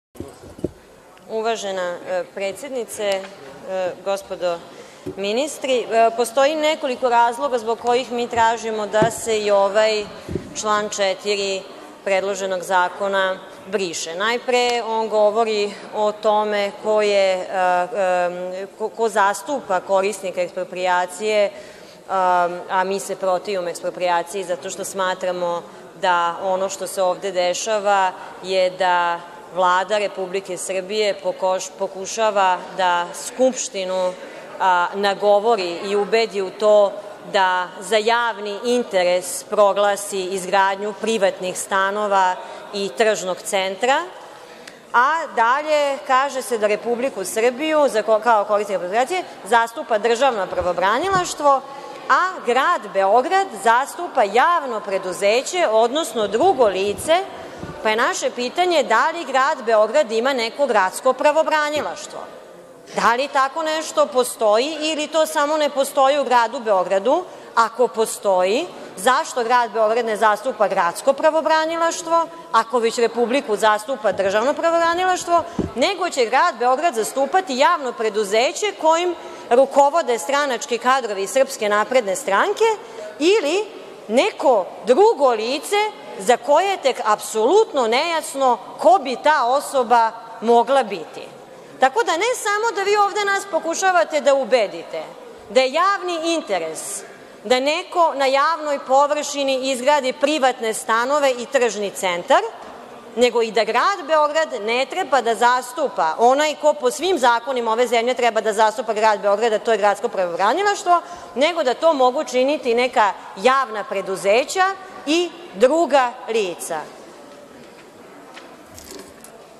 Aleksandra Jerkov, rasprava o amandmanima na leks specijalis o Beogradu na vodi
(Beograd, 9. aprila 2015) Narodna poslanica Demokratske stranke Aleksandra Jerkov u obraćanju na sednici Skupštine Srbije na kojoj se raspravlja o amandmanima na Predlog zakona o utvrđivanju javnog interesa i posebnim postupcima eksproprijacije i izdavanja građevinske dozvole radi realizacije projekta izgradnje "Beograd na vodi". (za više informacija posetite